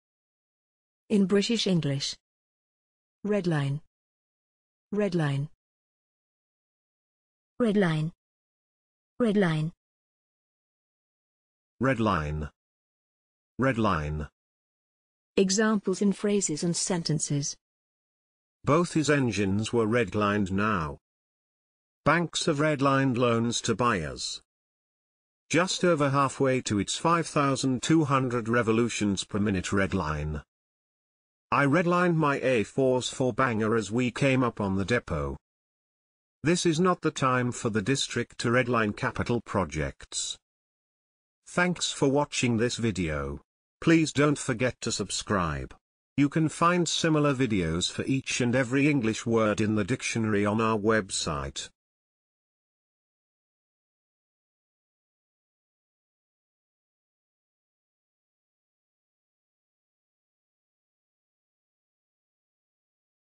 redline - pronunciation + Examples in sentences and phrases
This is the pronunciation of redline in British English + a few examples of its pronunciation in phrases and sentences.